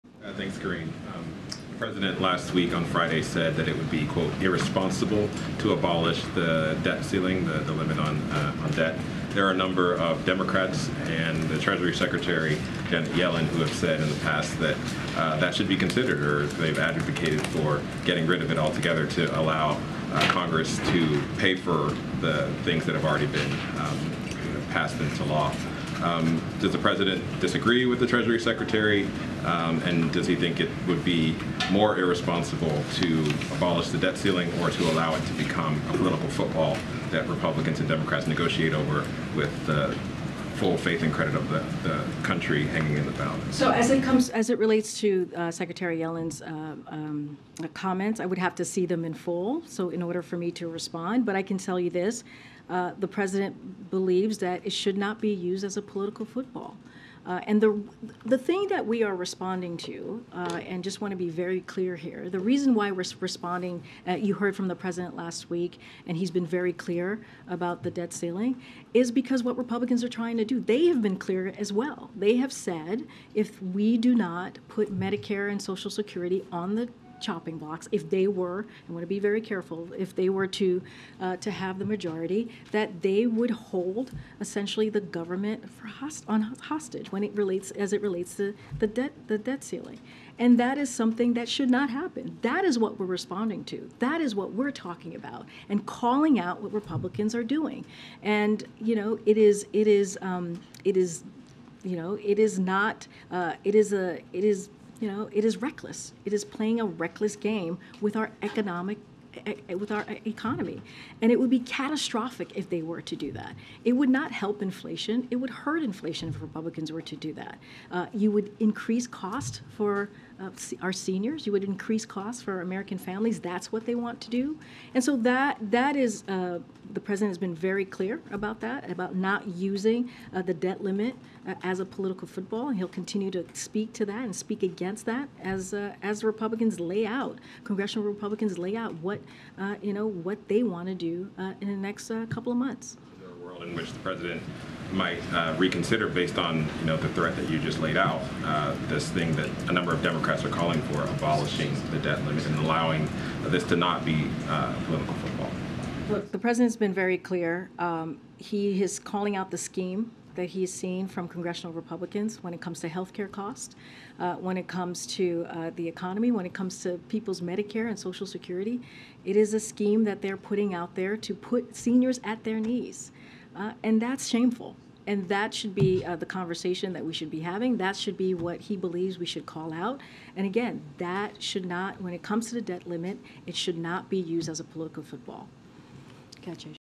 ‘It Is Reckless’: Karine Jean-Pierre Defends Biden’s Comments About Abolishing The Debt Ceiling
On Tuesday, Karine Jean-Pierre defended President Biden’s comments that abolishing the debt ceiling would be irresponsible.